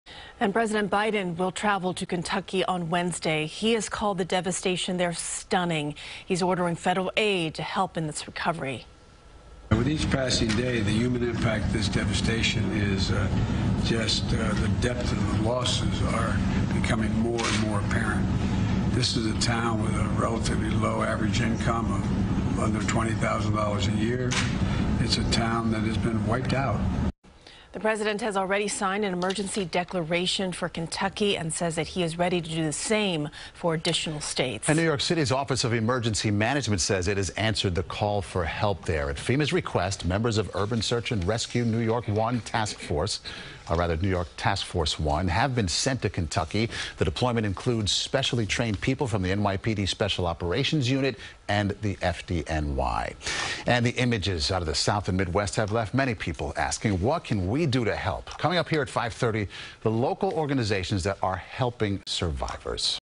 NYC Office Of Emergency Management Sends Search & Rescue Team To Kentucky
The deployment includes specially trained people from the NYPD Special Operations Unit and the fire department.